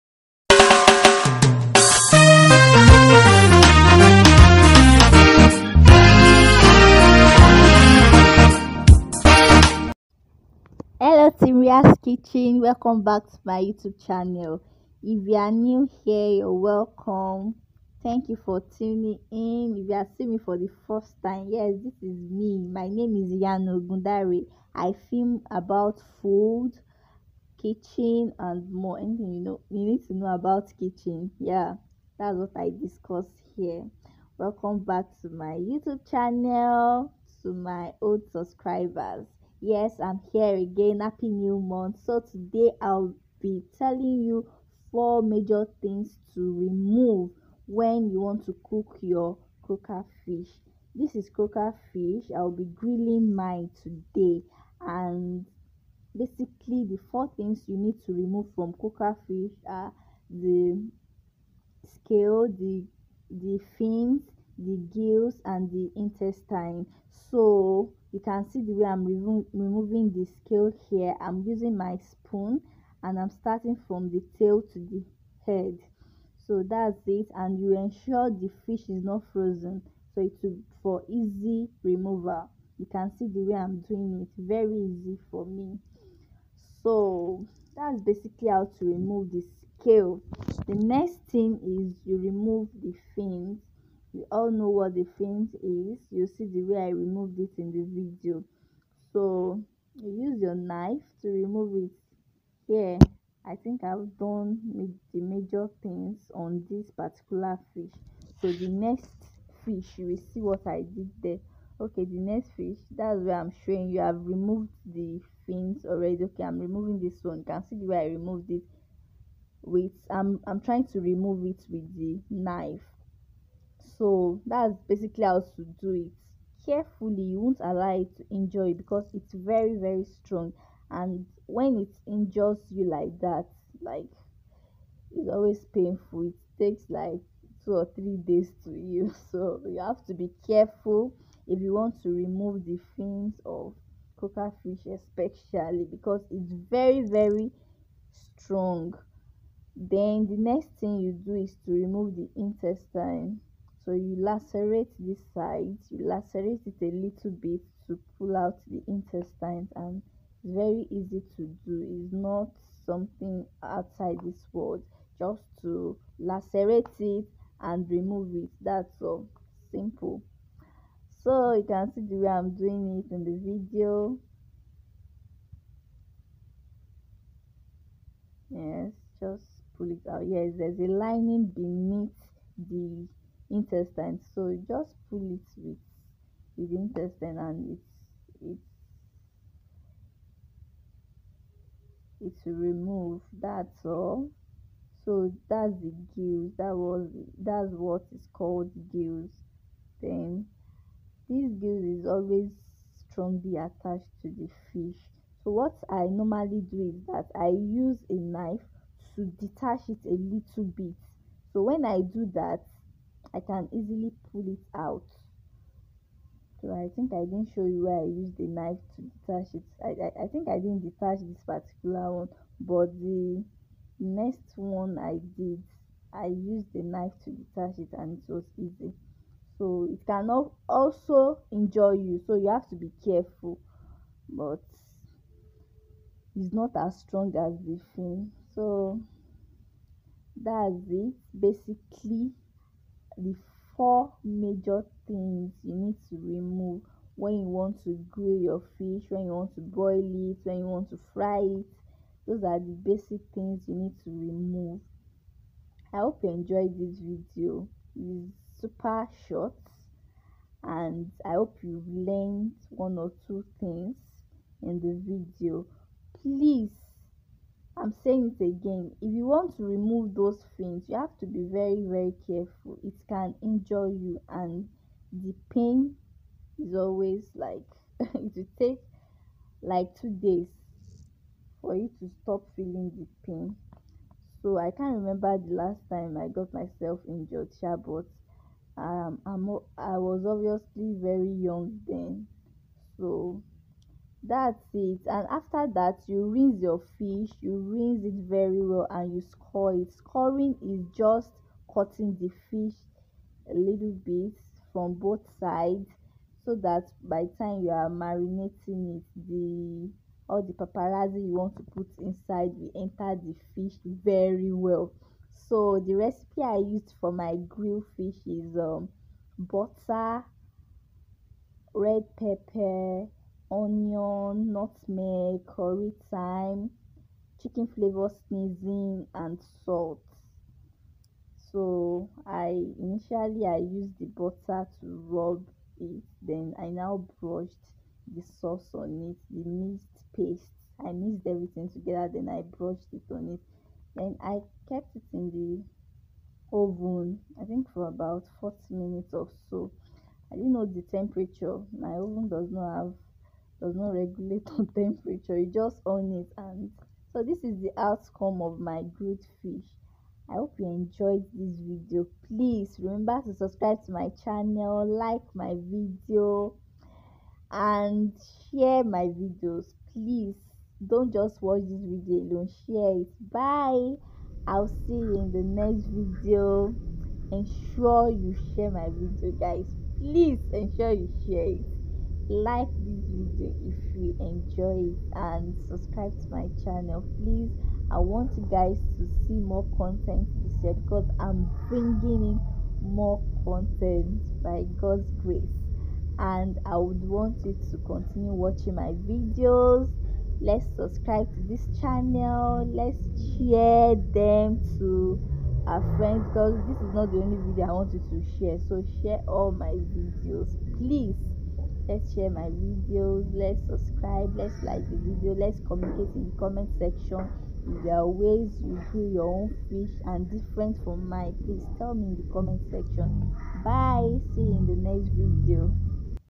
How to clean Croaker fish
Happy new year to all teamria's kitchen 🤗 💃
I demonstrated how to clean Croaker fish in this video.
The 4 major waste to remove are...
-scale
-fins
-intestines
-gills
And follow me on other social media..